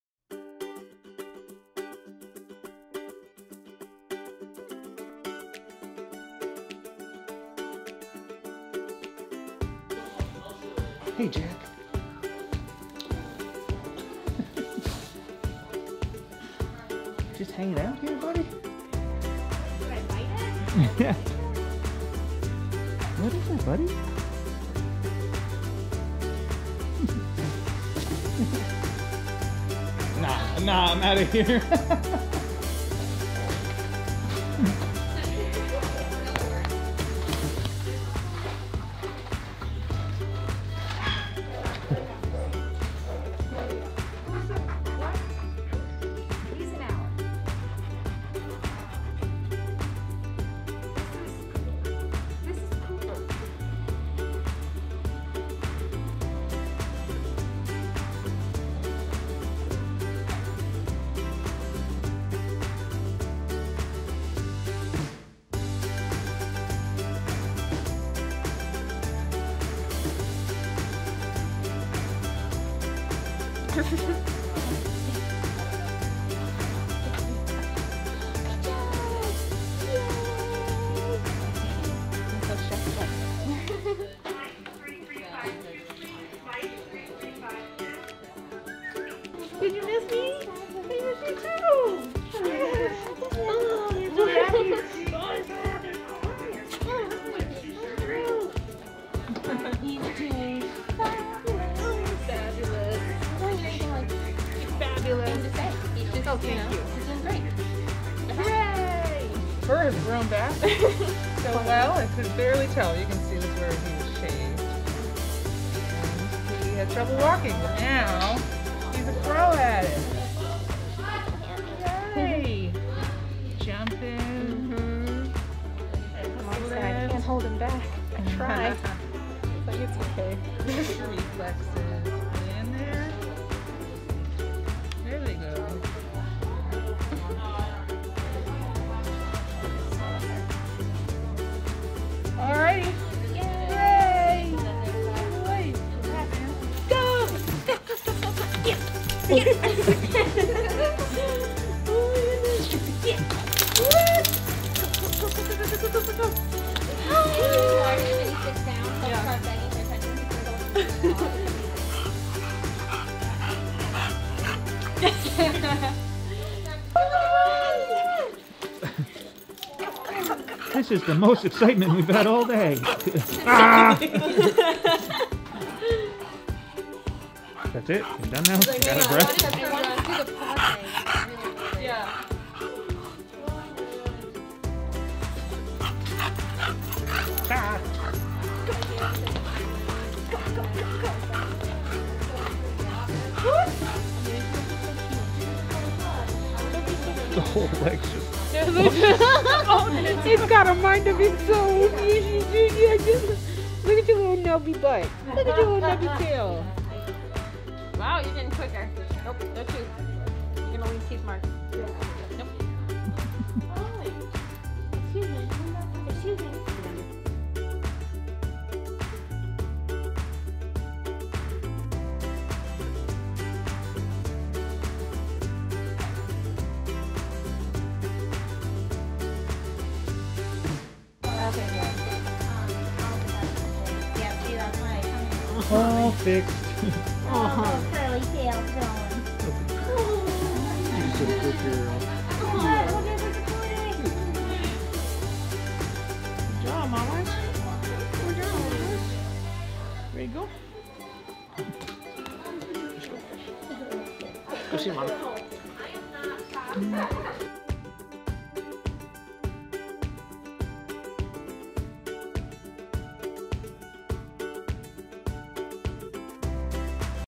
Episode 21: Top 5 Cutest Moments
*Warning: May make you smile and laugh. In this fun-filled recap episode, expect to see lots of cuddles, kisses, and hugs with our furry friends.